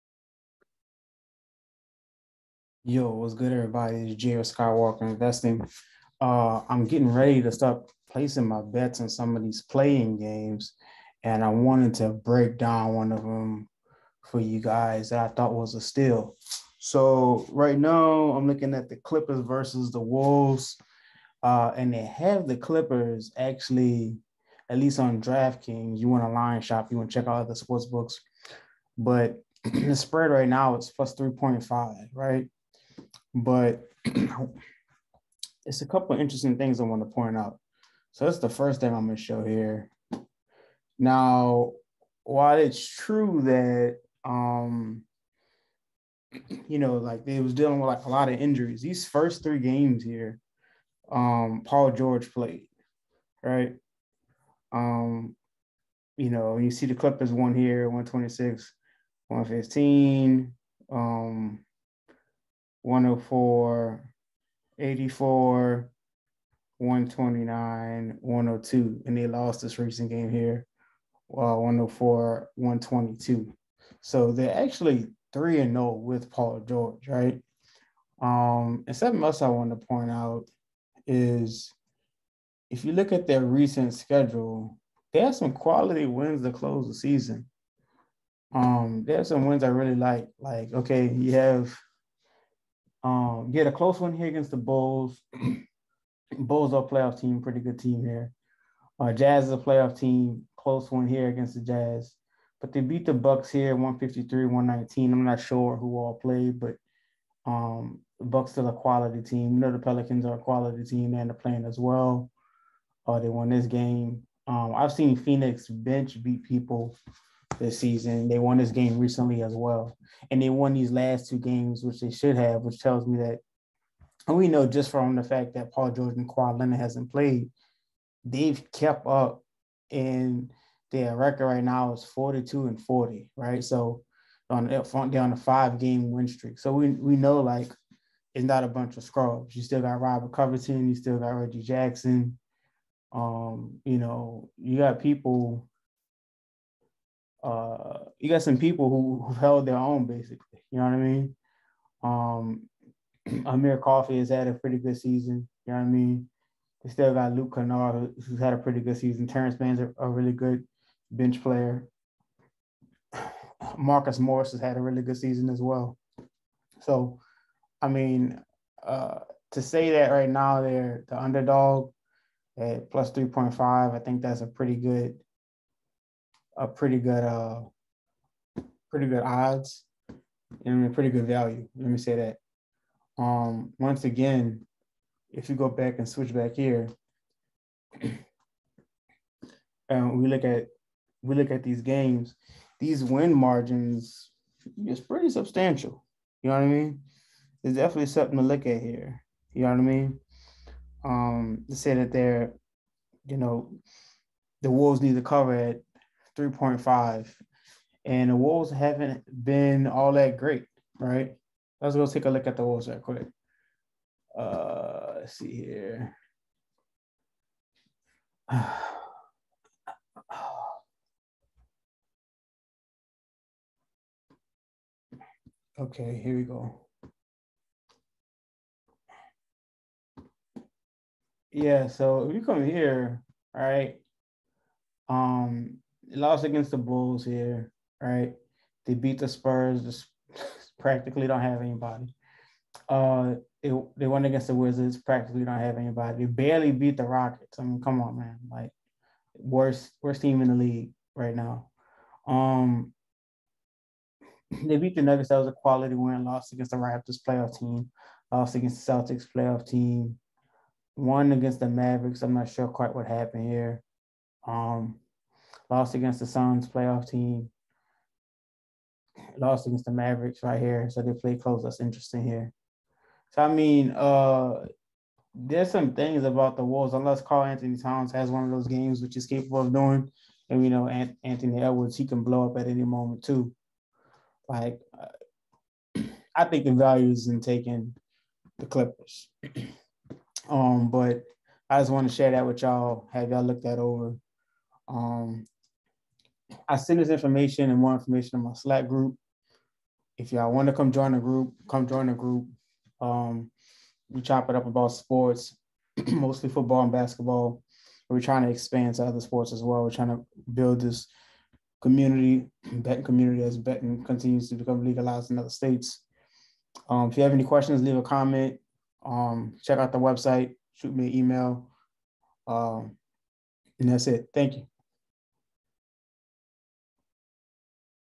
Clippers vs Timberwolves Play in Matchup
In this video we take a look at the betting lines on the NBA play-in game of the Clippers vs Timberwolves to see where the value is.